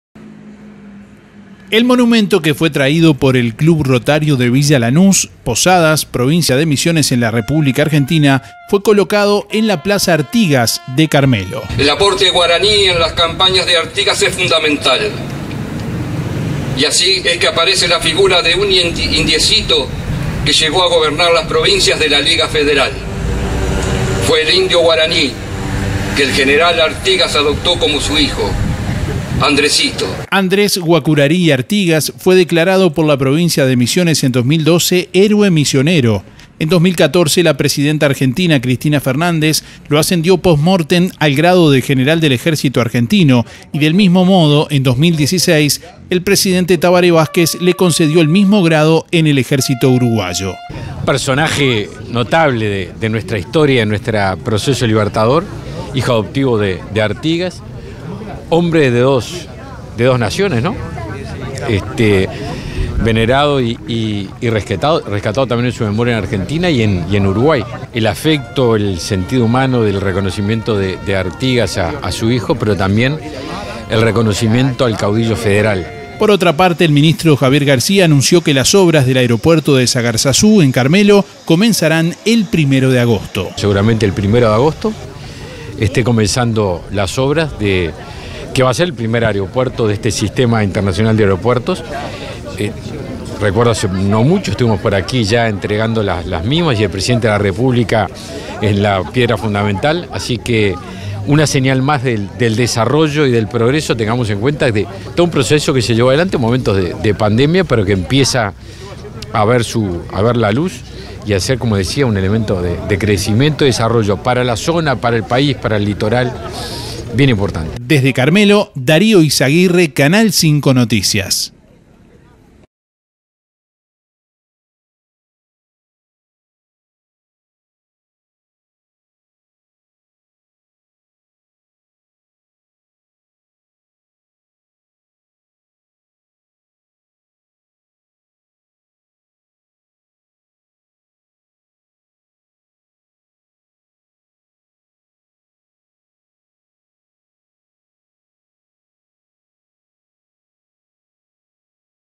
Reconocimiento a hijo adoptivo de Artigas en Carmelo
El Club Rotario de Carmelo inauguró un busto del General Andrés Guacurarí, hijo adoptivo del General José Gervasio Artigas. Del reconocimiento participó el Ministro de Defensa Nacional Javier García quién además anunció que las obras del Aeropuerto de Zagarzazú en Carmelo comenzarán el 1 de agosto.

El monumento que fue traído por el Club Rotario de Villa Lanús, Posadas, Provincia de Misiones en la República Argentina fue colocado en la Plaza Artigas de Carmelo.

Andrés Guacurarí y Artigas, fue declarado por la provincia de Misiones en 2012 “Héroe Misionero”. En 2014 la Presidenta Argentina Cristina Fernández lo ascendió post mortem al grado de General del Ejército Argentino y del mismo modo en 2016 el Presidente Tabaré Vázquez le concedió el mismo grado en el Ejército Uruguayo.

Por otra parte, el Ministro Javier García anunció que las obras del Aeropuerto de Zagarzazú en Carmelo comenzarán el 1 de agosto.

Así lo informó para Canal 5 desde Carmelo, Darío Izaguirre